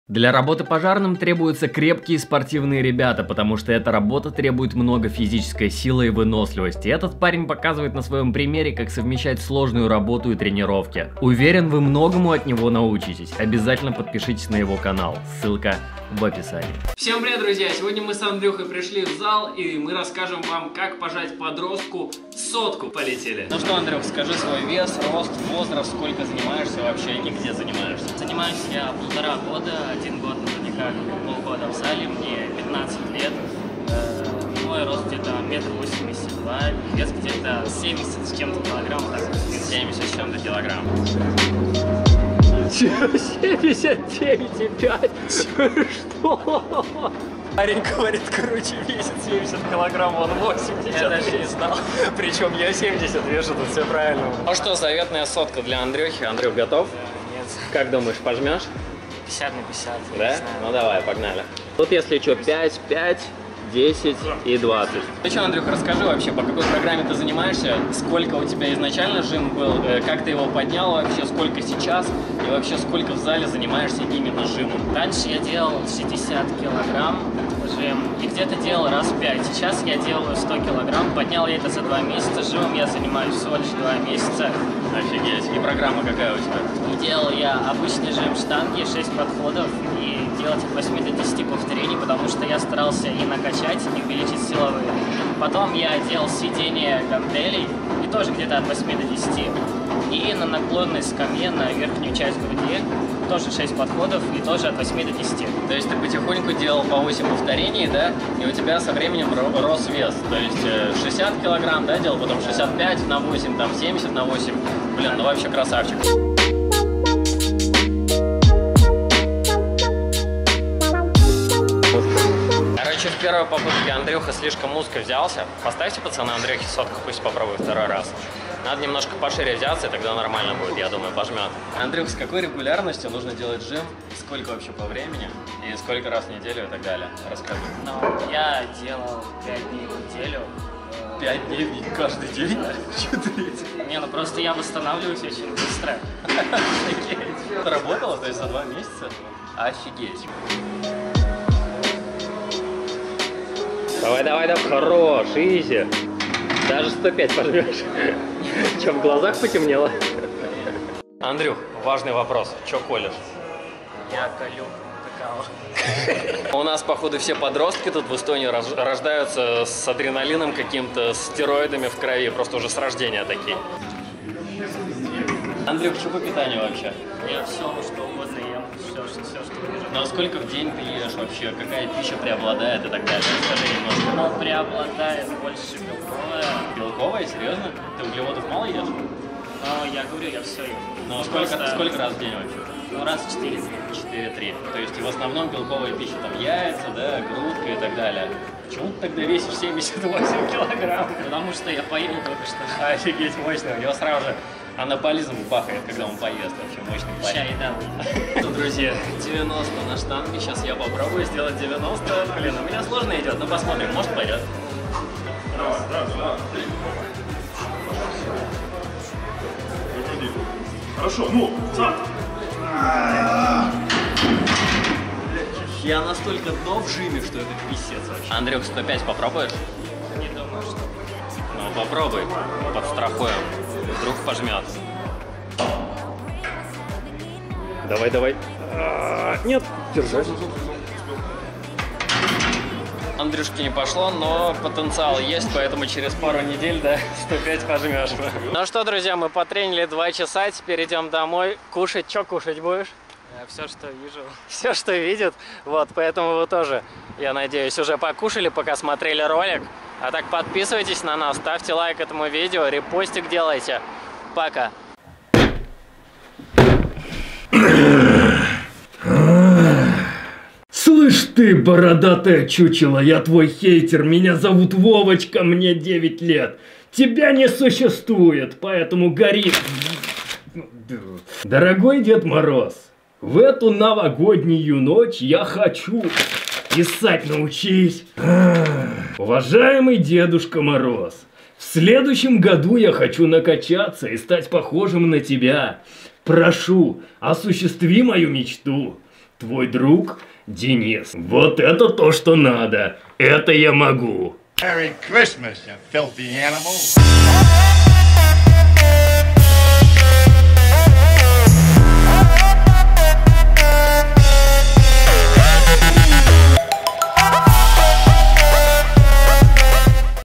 Прибавил к ЖИМУ +40кг за 2 Месяца!
В этом видео вы увидите как подросток за 2 месяца прибавил к своему жиму лежа 40кг! С 60кг дошел до 100кг всего за 2 месяца! Также увидите его тренировочную систему и силовые показатели.